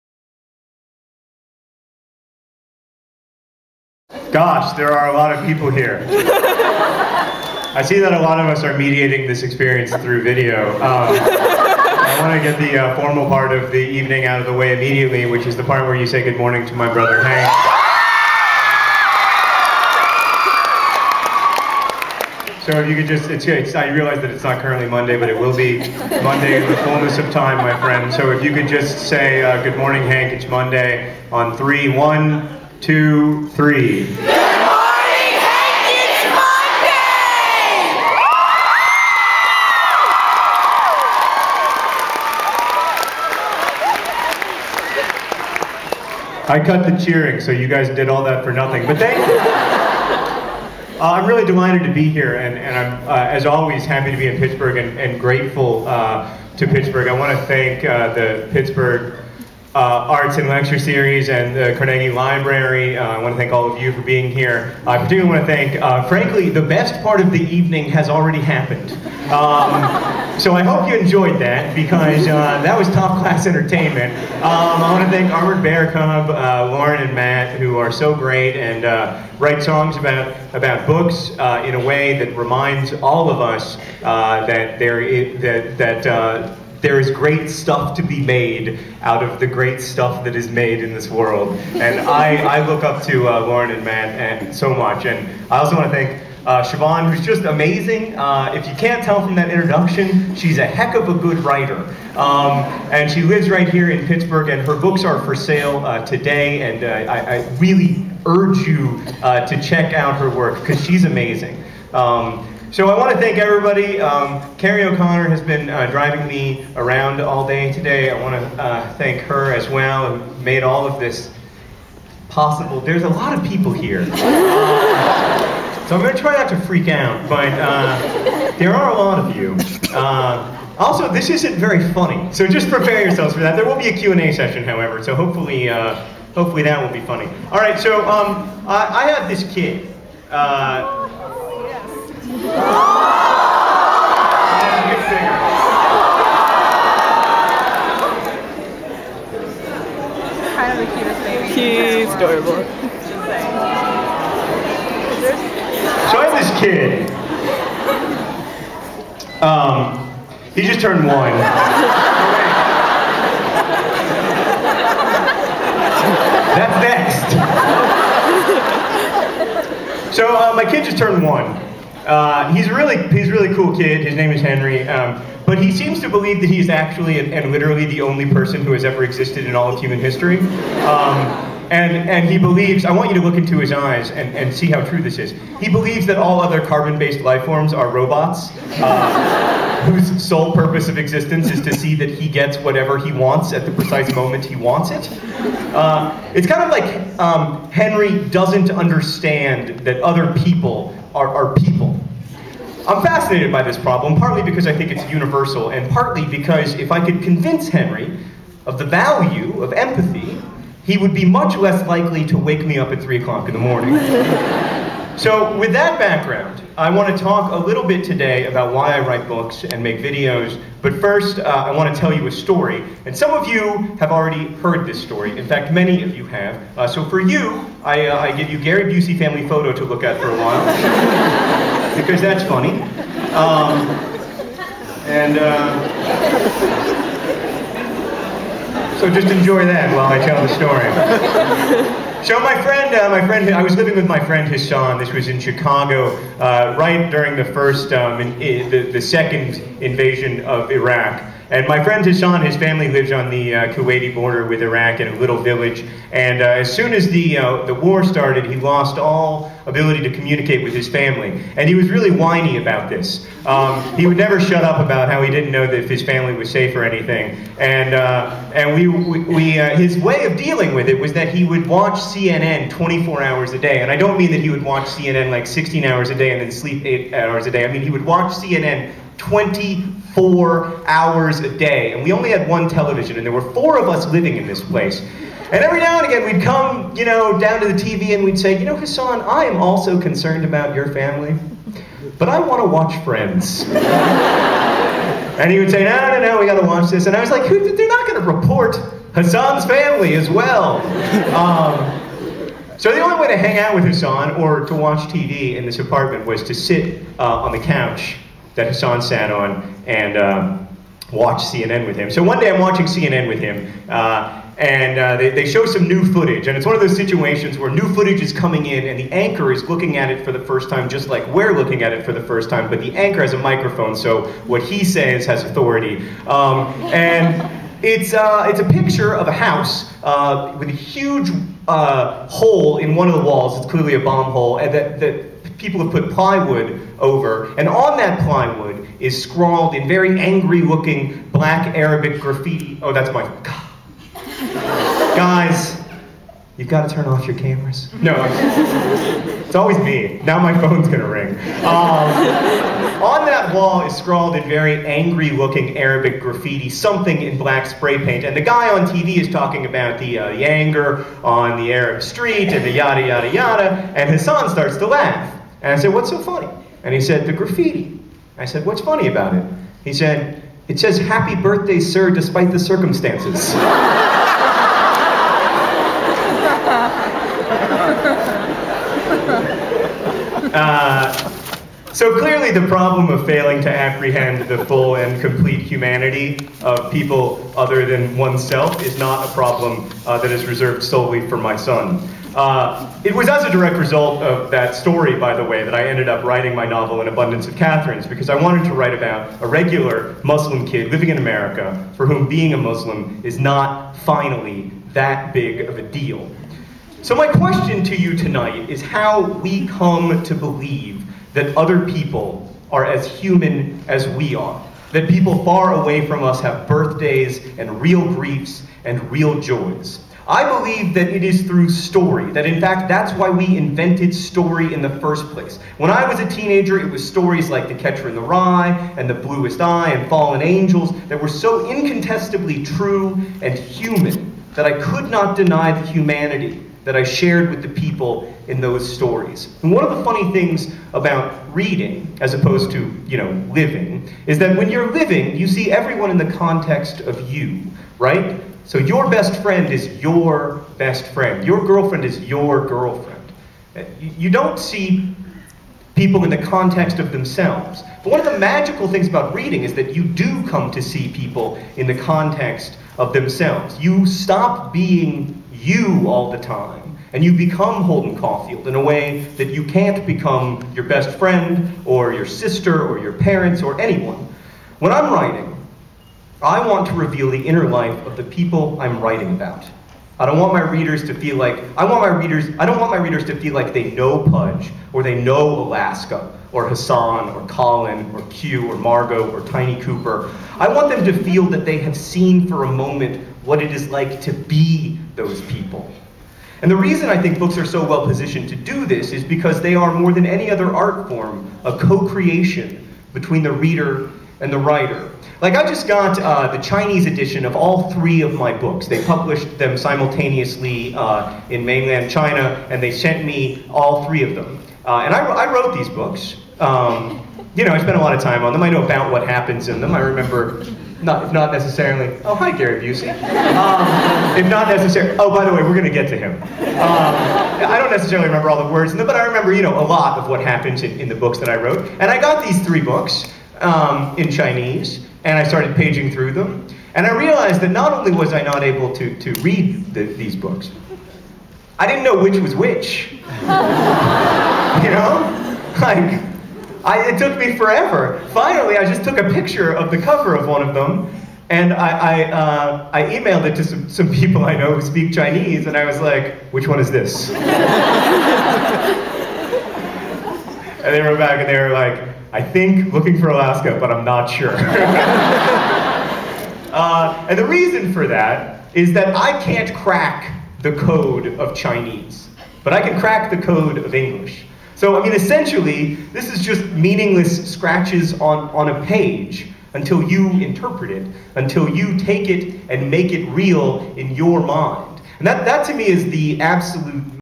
John Green's Lecture Part I
The Irregulars went to see John Green speak at the library. It was made of awesome.

Part 1